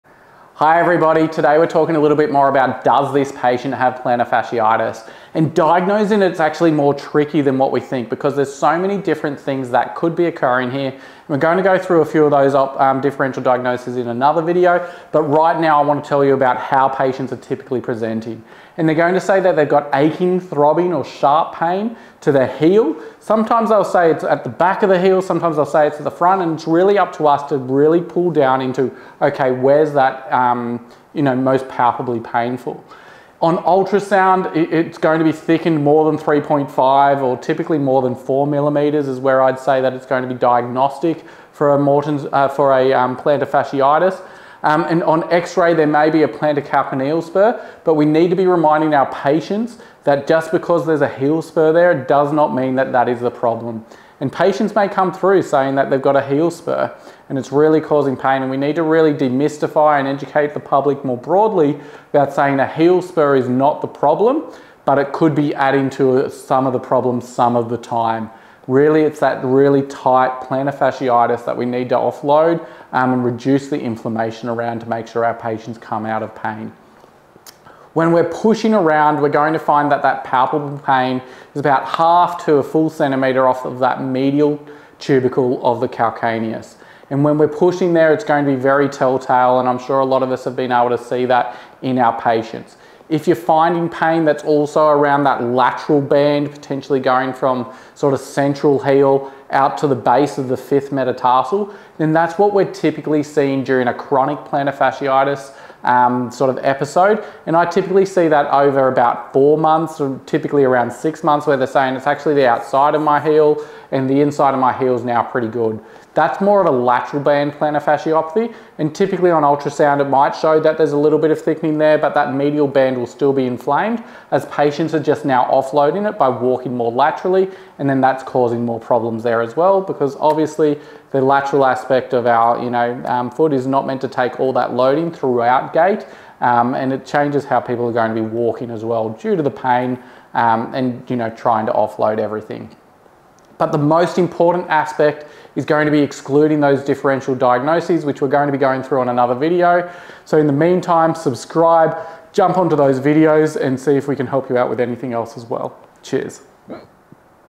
Does This Patient Really Have Plantar Fasciitis? | Exploring Heel Pain Causes
Does This Patient Really Have Plantar Fasciitis?

In this video, we take a closer look at whether this patient’s heel pain aligns with a diagnosis of plantar fasciitis or if another condition could be at play. 🩺 Heel pain can stem from various causes, making accurate diagnosis crucial.

💡 **Key Symptoms Discussed:**

✅ Aching, throbbing, or sharp pain: Classic signs often reported by patients experiencing plantar fasciitis.
✅ Ultrasound findings: A thickened plantar fascia of 3.5mm may indicate inflammation or stress.
✅ X-ray evidence: The presence of a plantar calcaneal spur, a bony growth near the heel, can sometimes be associated with chronic plantar fasciitis.
✅ Palpable pain: Localised tenderness approximately 1cm off the medial calcaneal tubercle is a hallmark sign of this condition.

We also discuss the importance of excluding differential diagnoses such as nerve entrapments, fat pad atrophy, or stress fractures, which may mimic the symptoms of plantar fasciitis.

Join us as we dive into the diagnostic process, helping you better understand this common but sometimes misunderstood condition. 🦶 Whether you’re a healthcare professional or someone seeking clarity on heel pain, this video offers valuable insights.